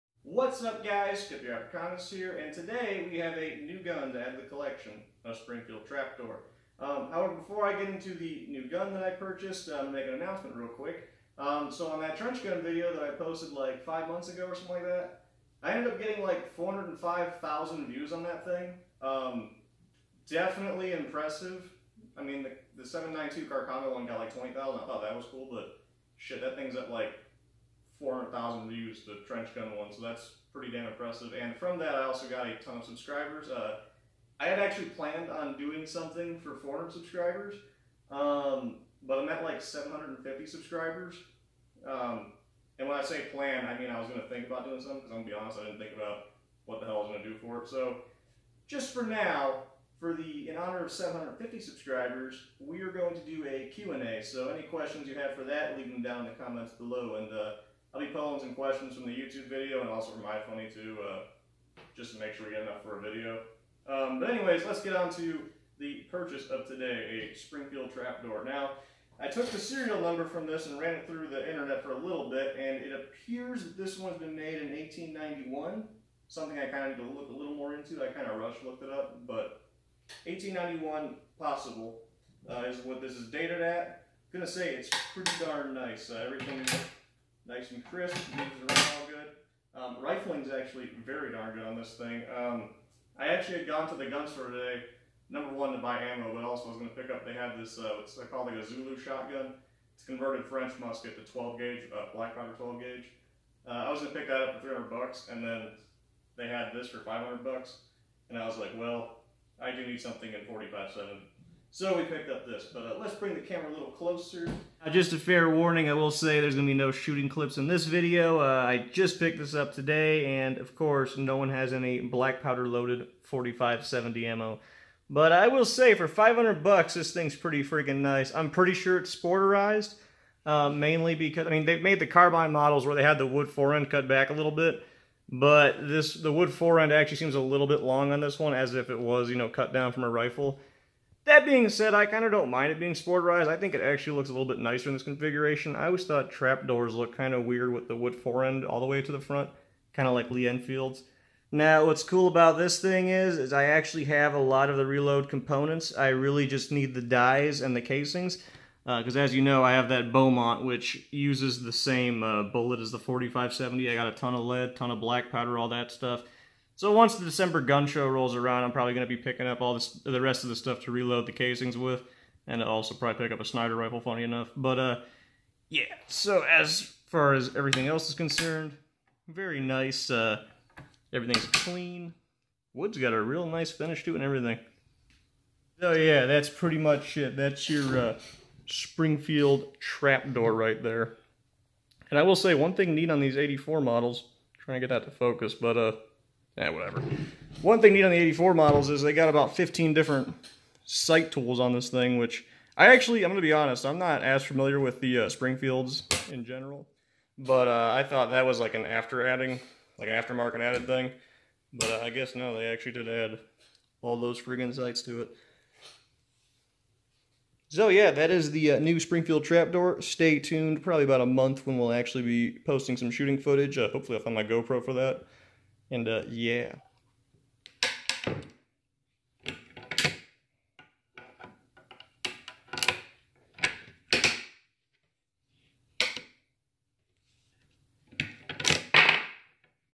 Trapdoor Acquired
Picked up this nice Springfield Trapdoor for like 500 bucks. Figured I'd do a show and tell. Don't forget to leave your questions for the QandA down in the comments.